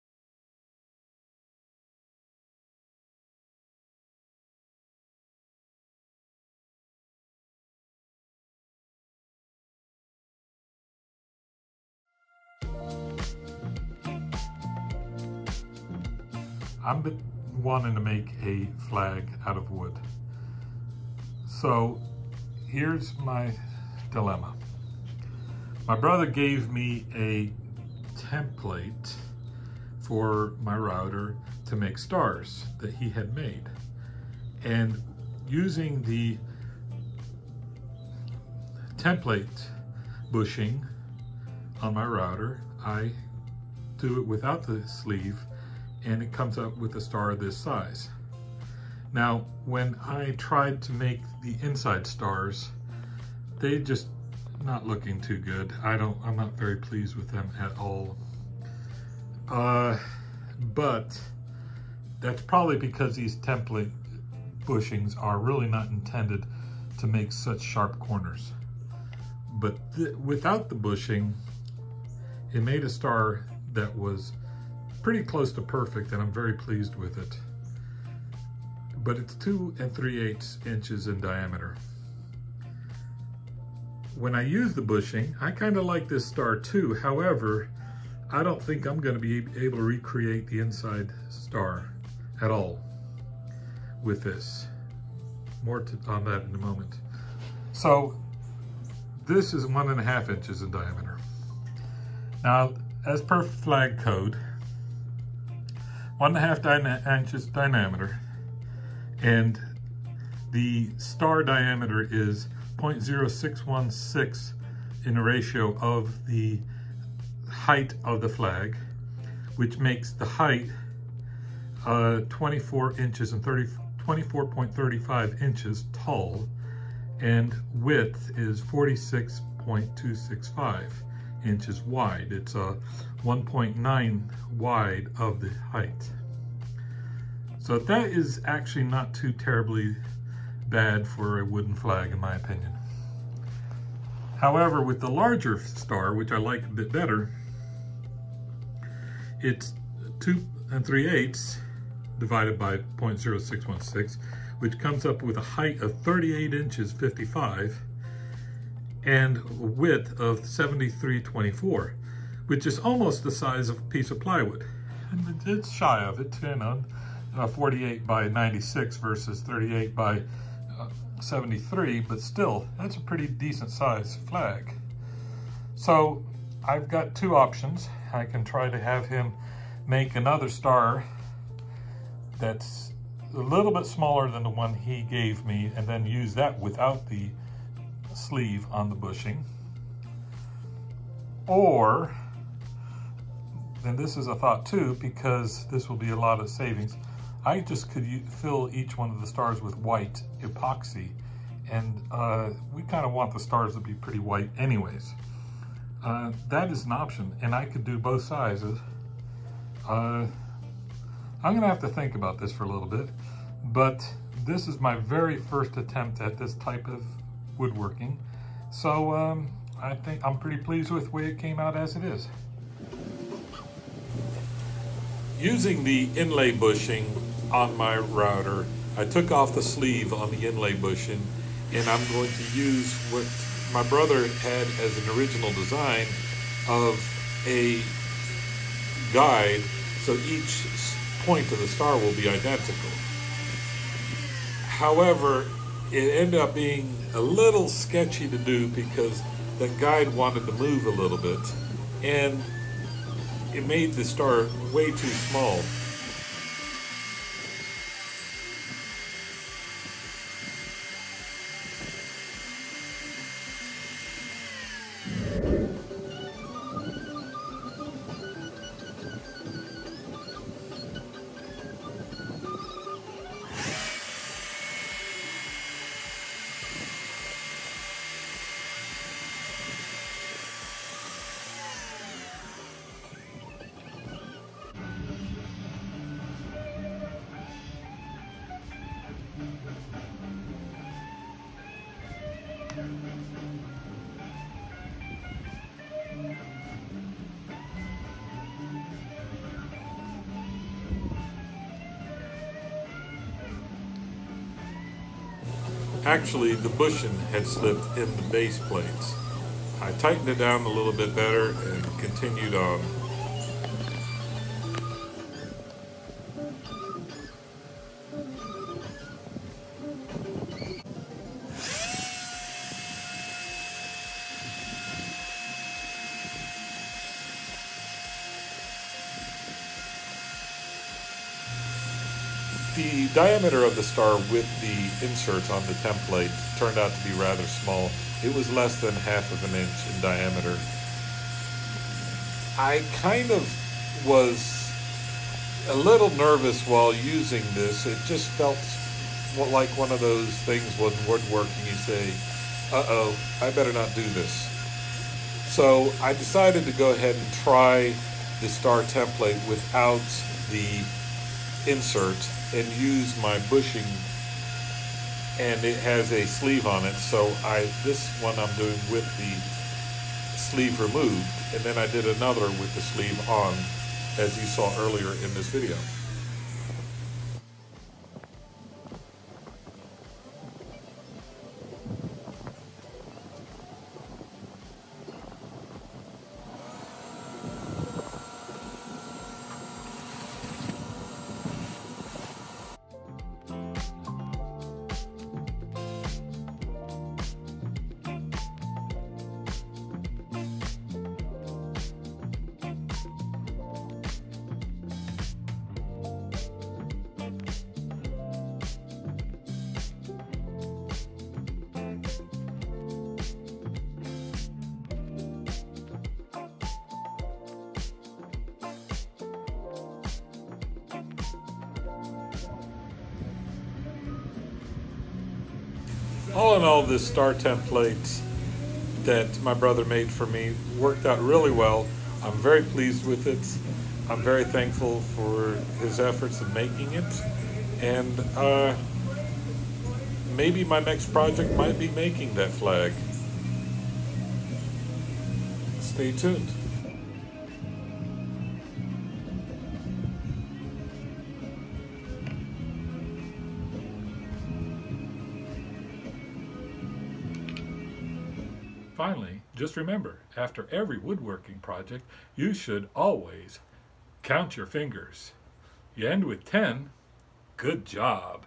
Router inlay star
This is the very first attempt I have ever made of making inlay using the router. It came out rather good for a very first attempt in my opinion. The real trick is move slowly and don’t push too hard on the router as you’re doing this. I will be using this technique again￼.